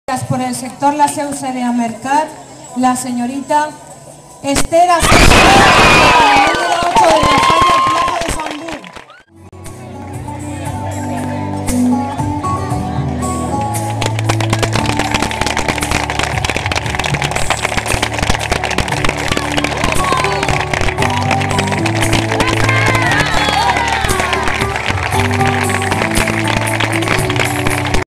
Señorita Ester Asensio Ferrando
La señorita ester asensio ferrando es de la falla SANT BULT sector la seu-xerea-el mercat